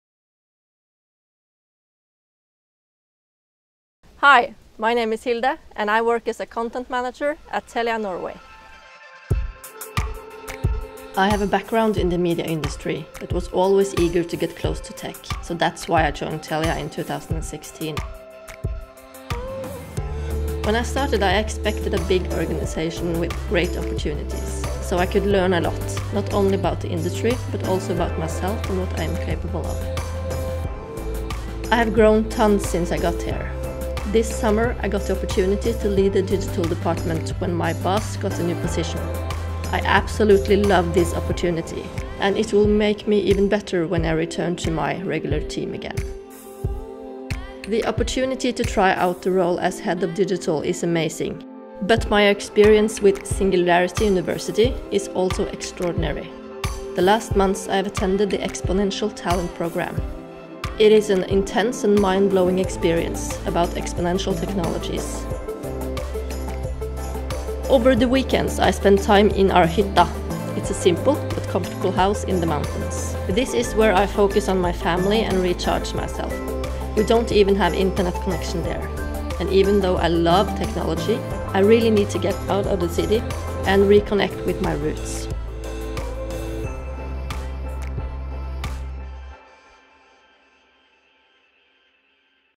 Møt folka våre: Hilde Aasnæs
Hilde startet i Telia i 2016, og jobber som Content Manager i digitalavdelingen. Les mer om folka våre her